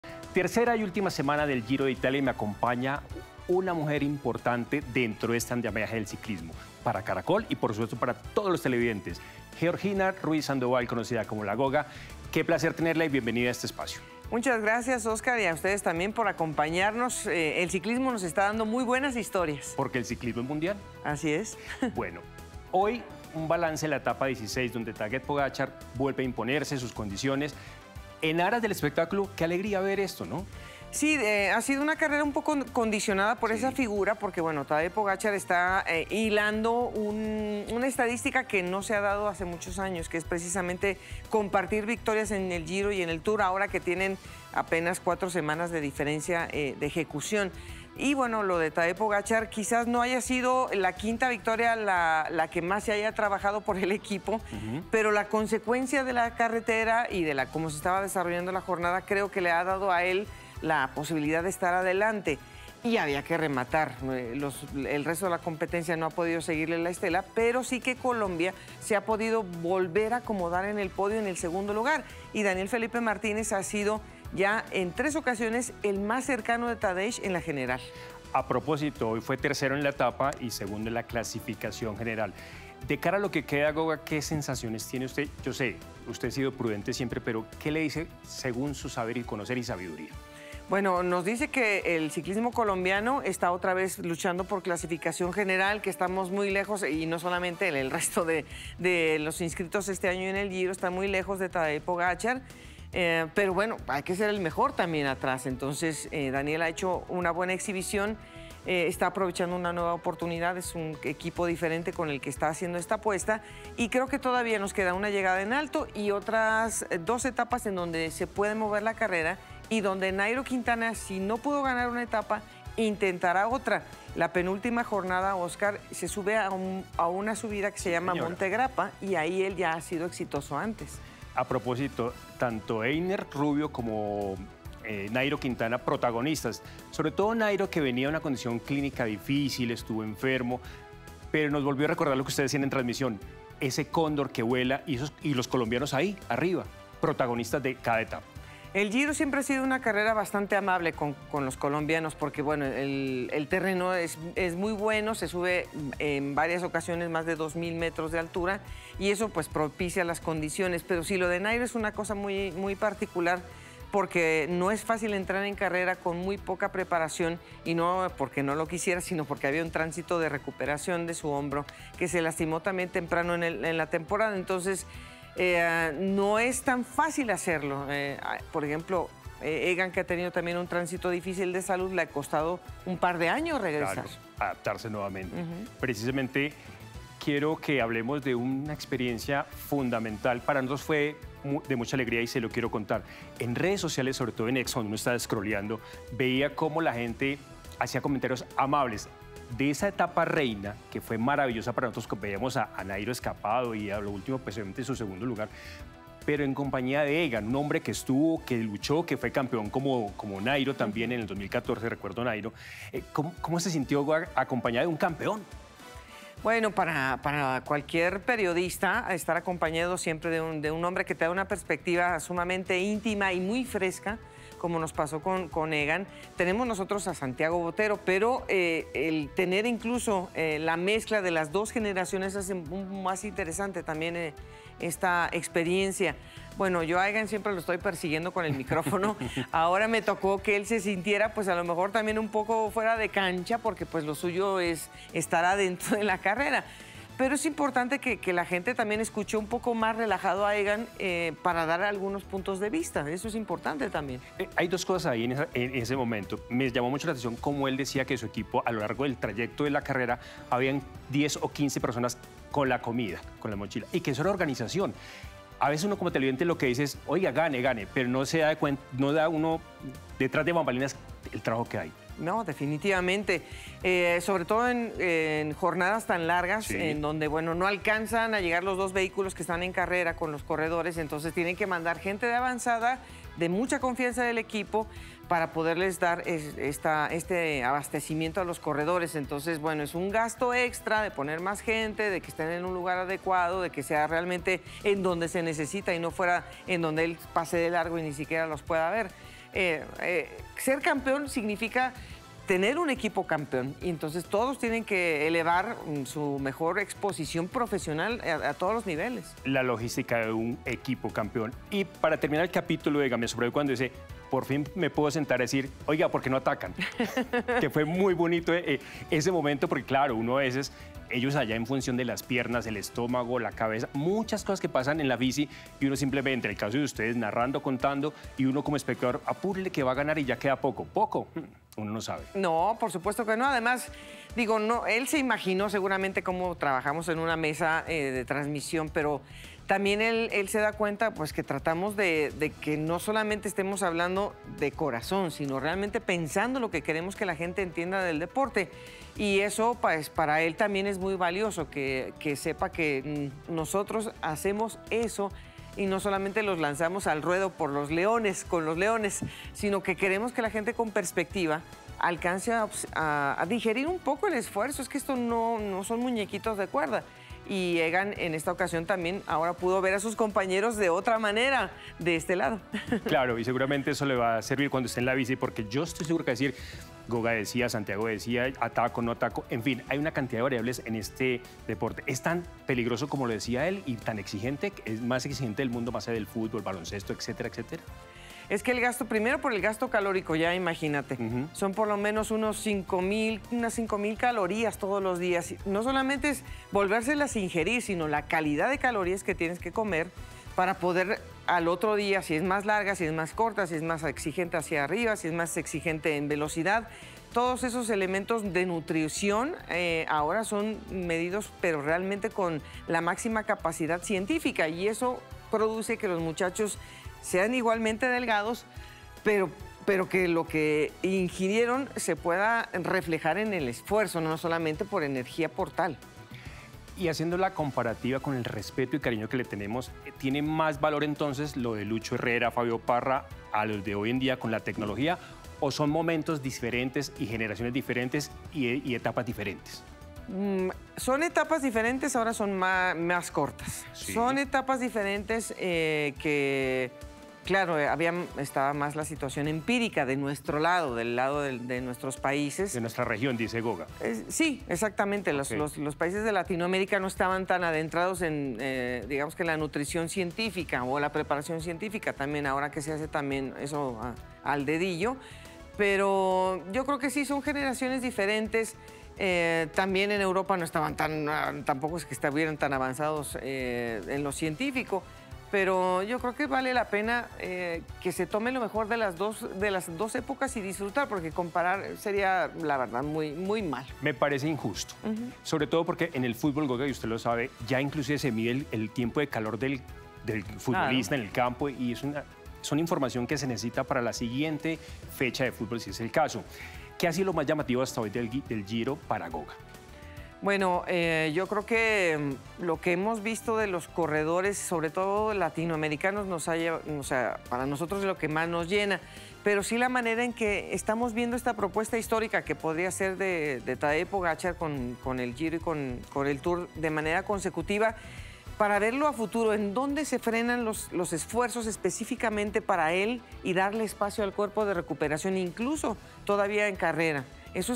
Giro de Italia 2024 en la recta final, tercera semana y análisis con una especialista: "Goga"
El Giro de Italia la primera de las tres grandes carreras por etapas en el mundo parece tener un campeón anticipado,  además los ciclistas colombianos protagonistas y animadores de esta fiesta ciclística.  De esos ítems nos ocupamos con nuestra especialista en la materia, Giorgina Ruiz Sandoval "Goga" quien hace parte del Team Caracol de Ciclismo.